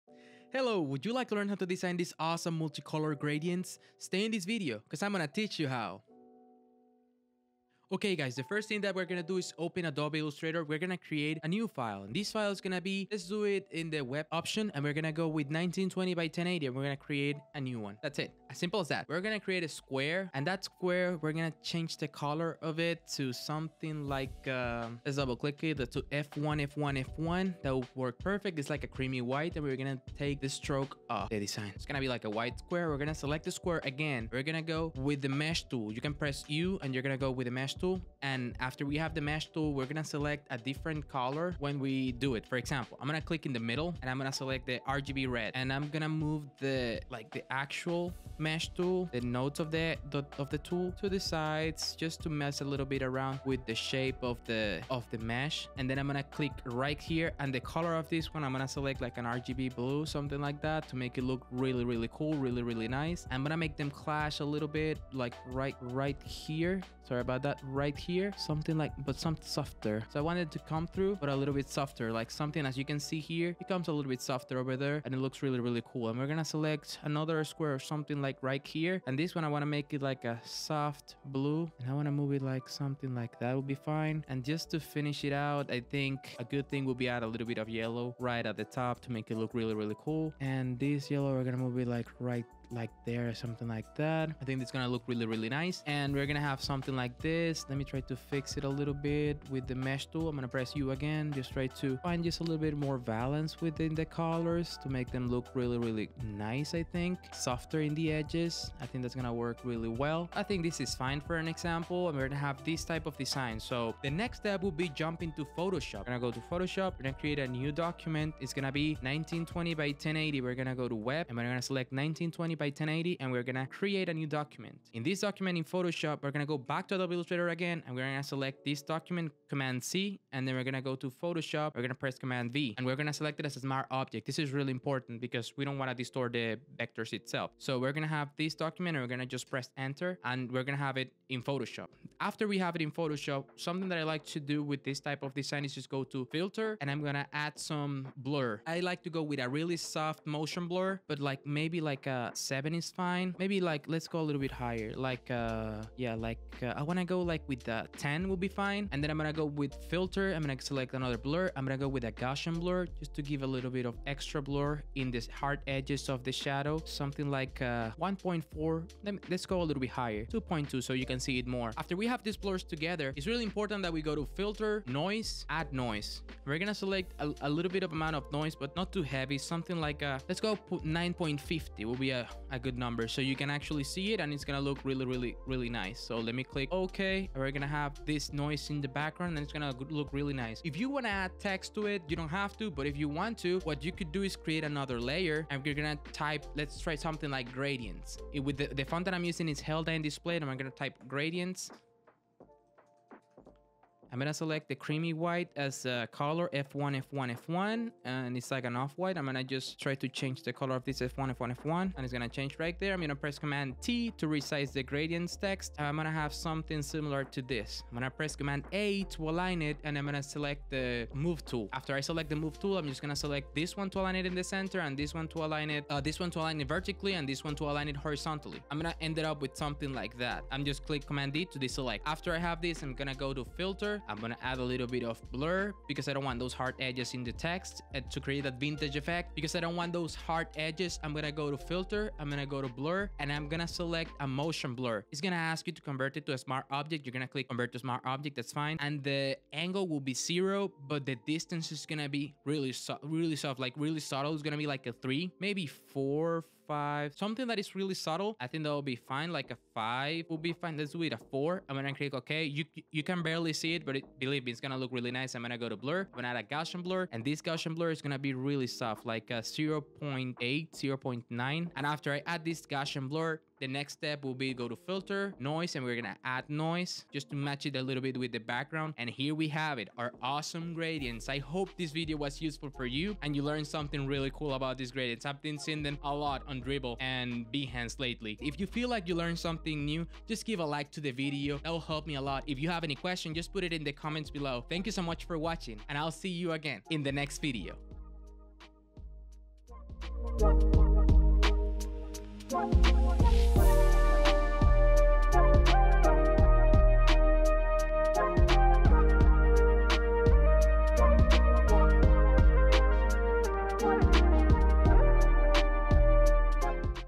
Multi-Color Gradients Using Adobe Photoshop & Illustrator // Tutorial 2022
Learn how to create Multi-color gradients using adobe photoshop and Illustrator cc2022 using the gradient mesh tool to create the gradient in illustrator and adding custom textures in photoshop.

As a bonus we will use some adjustment layers to make the design even more interesting!
in fact I will teach you about the use of photoshop effects with some adjustment layers and more... this will help you make that design more appealing However One of the most important things to learn in graphic design is to learn to manage layout elements with great spacing, proportion, rhythm, and harmony.

0:00 Intro
0:23 Illustrator Design
2:00 Photoshop
3:15 Adding Text
4:44 Final Design

In graphic design is extremely important to learn about new trends and how and how to apply them in your designs to have a better approach generally in your everyday designs. if you have any questions about how to work the gradient mesh, how to use the adjustment layers, or if you just simply wanna say hi Just add it in the comments below and I will try my best to help you understand.